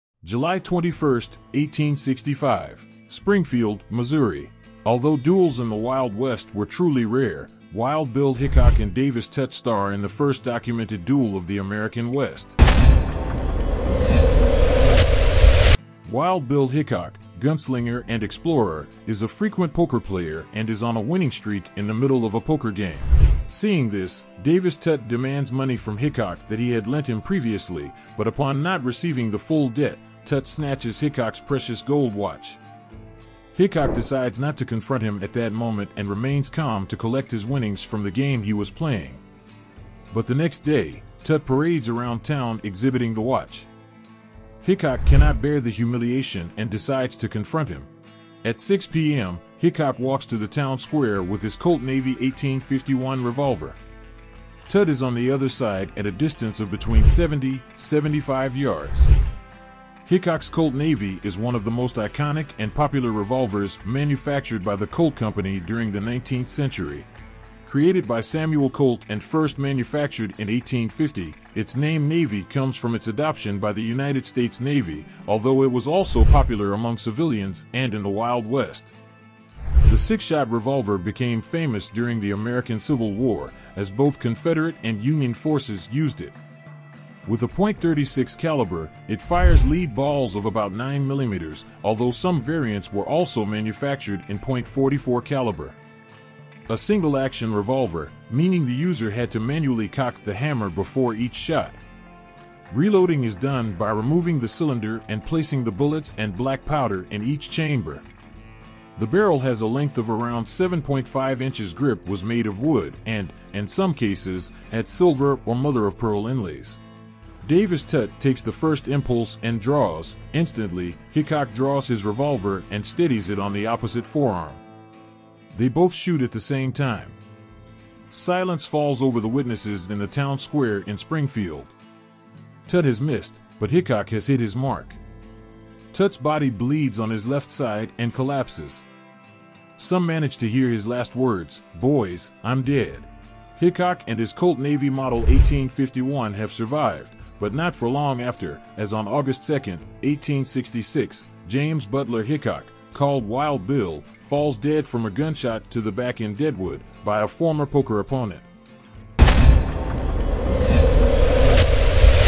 The incredible story of the fight for a watch
The duel between "Wild Bill" Hickok and Davis Tutt is considered one of the few documented duels between gunfighters in the Old West.
Find out what weapons were used and how it ended.
NOTE: Bill Hickok's date of death is 1876.